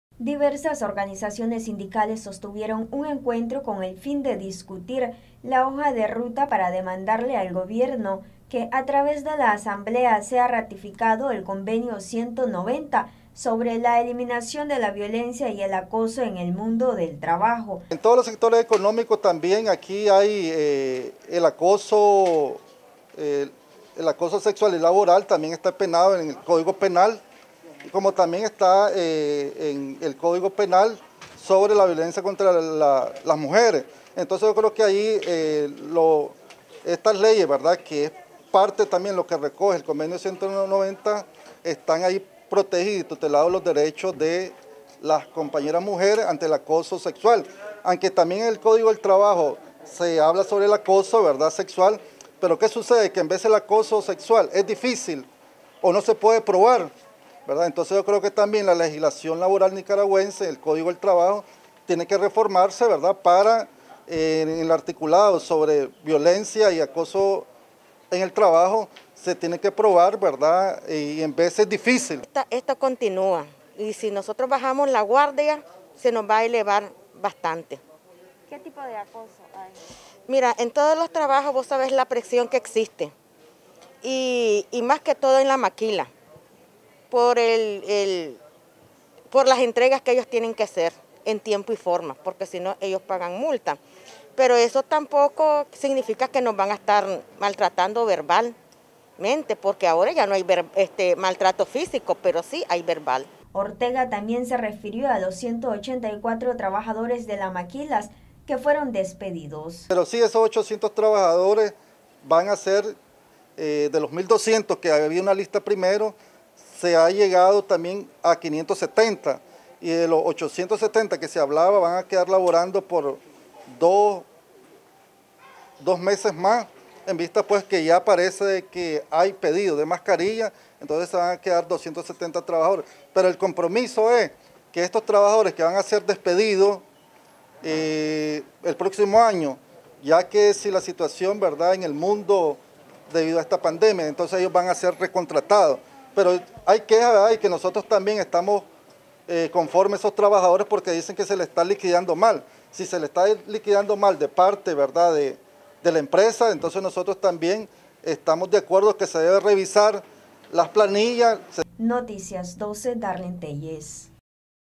Organizaciones sindicales solicitan al gobierno ratifique el convenio 190
Consenso entre organizaciones sindicales están solicitando que a través de la asamblea se ratifique el convenio 190, este consiste en la eliminación de la violencia y acoso en las empresas.